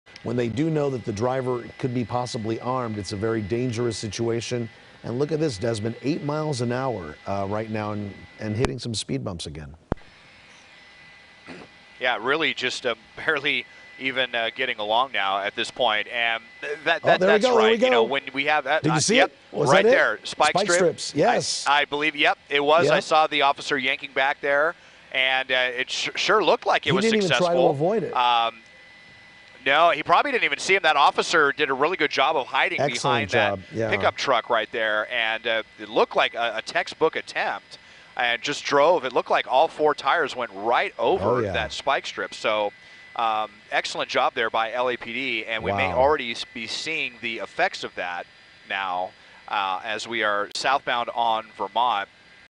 Officer throws out spike strip right in front of suspect
An officer threw a spike strip right as the suspect drove by.